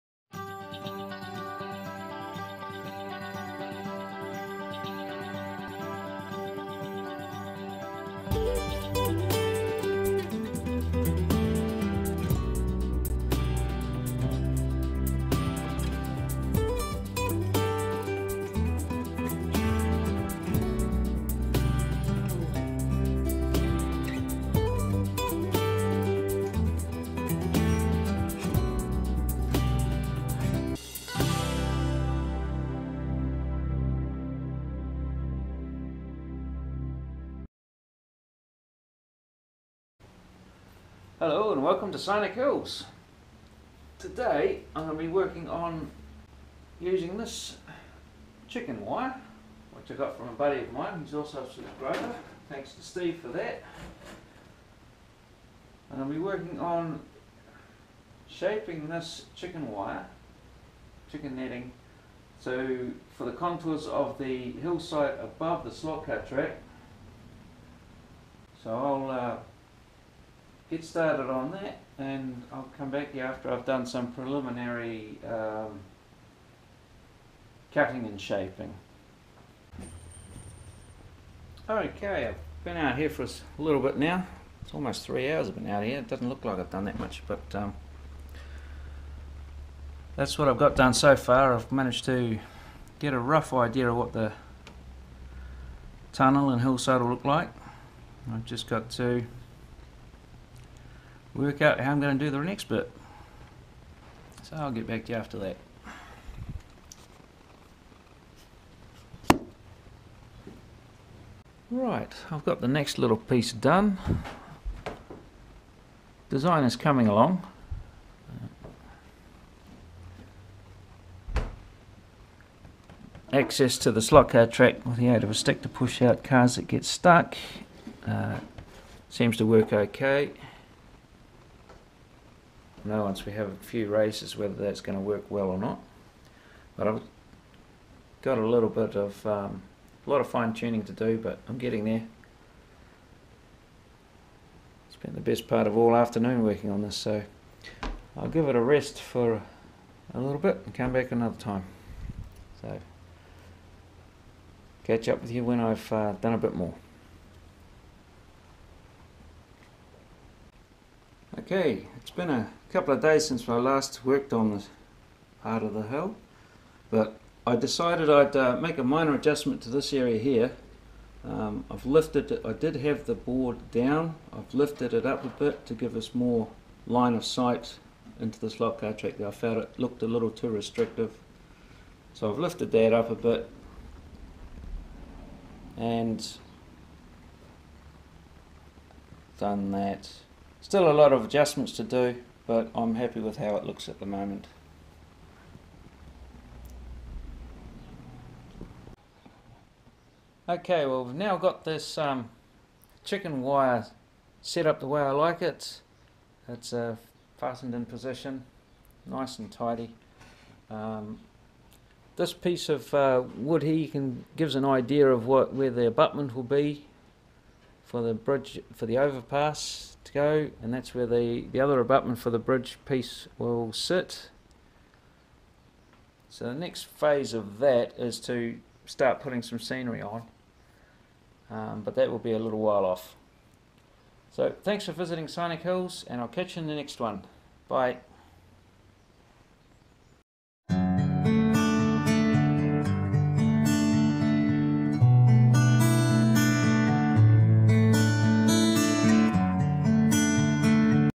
Slot car layout. Adding structure to downhill section of track
In this episode, we add some structure for the hillside and tunnel on the downhill section of the track